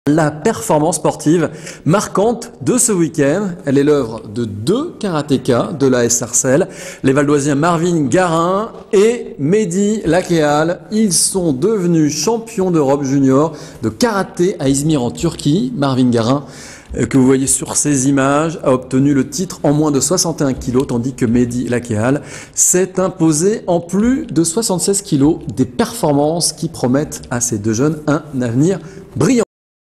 2 karatékas de Sarcelle champions d'Europe !
Medhi Lakhehal et Marin Garin de Sarcelles, sont tous 2 devenus champions d'Europe juniors en karaté. L'un en catégorie +de 76kg et l'autre -de 61kg. (DROITS RESERVES . Pour toute exploitation sur TV, web, mobile, aller